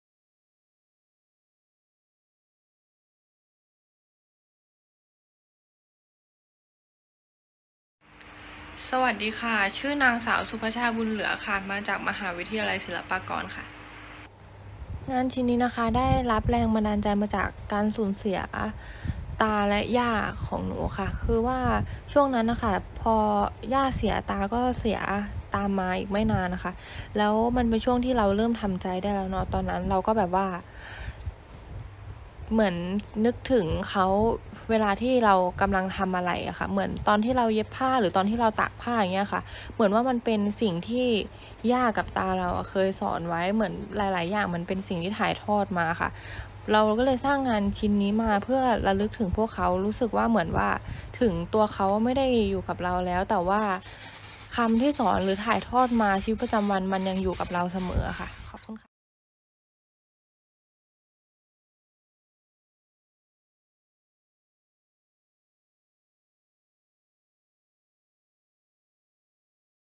White Canvas 2021 " Always " by สุภัชชา บุญเหลือ
WHITE CANVAS Thailand 2021 — เป็นงานที่ได้แรงบันดาลใจมากจากการสูญเสียของตาและย่า เป็นขณะที่อยู่ในช่วงทำใจได้ในระดับหนึ่งแล้ว ซึ่งเราตั้งใจทำงานชิ้นนี้ขึ้นมาเพื่อระลึกถึงพวกท่าน ในตัวงานจะให้ความหมายว่าเขายังอยู่กับเราเสมอผ่านชีวิตประจำวัน สิ่งที่เราทำ เช่นเย็บผ้า ตากผ้า หรือสุนัขที่เลี้ยงด้วยกัน ตัวงานบ่งบอกว่าถึงแม้วันนี้ตัวพวกท่านจะไม่ได้อยู่กับเราแล้ว แต่ท่านก็ยังอยู่กับเราเสมอผ่านคำสอน ผ่านสิ่งที่ถ่ายทอดมาและหลอมรวมเป็นตัวเราในปัจจุบันนี้